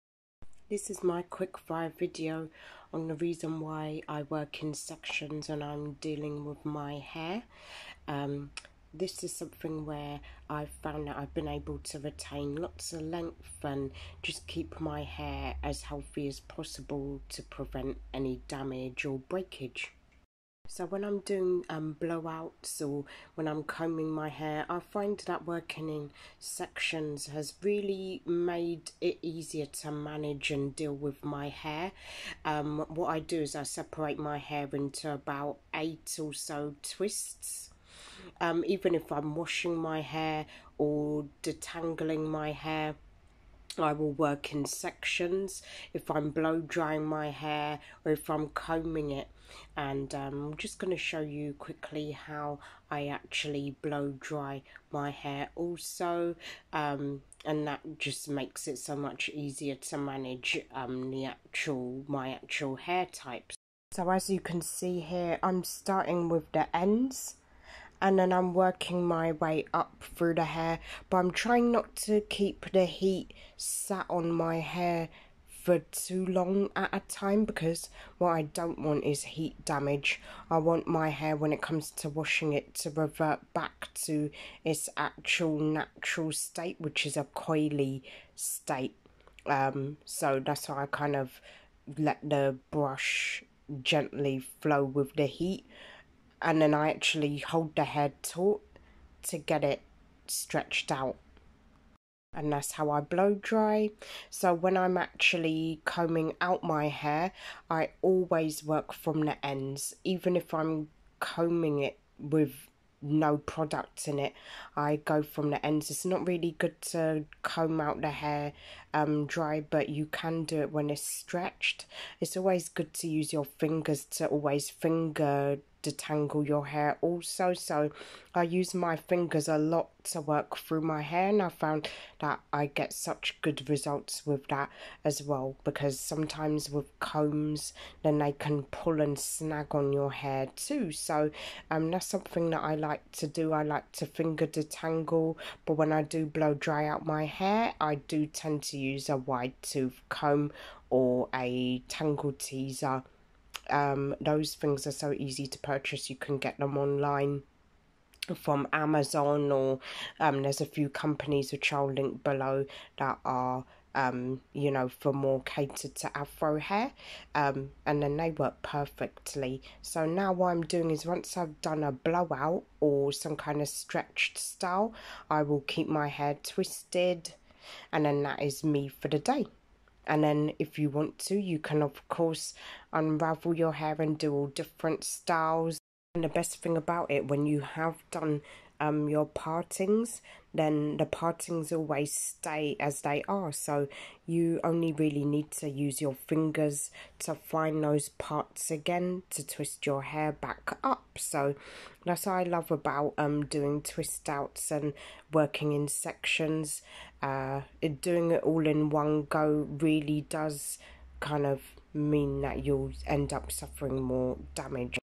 My tips best tips on how I maintain my fine 'NATURAL HAIR' 26 July 2022
This short is how I have maintained the length of my Fine Natural Hair, my quick simple tips to fine natural hair growth

Have you recently started a natural hair journey, if so what are your experiences?

DM Me in regards to the Derma roller and also

Biodegradable Eyebrow Razors/Multipack 12/ Dermaplanning for Women and Men/Facial Hair Removal & Eyebrow Shapers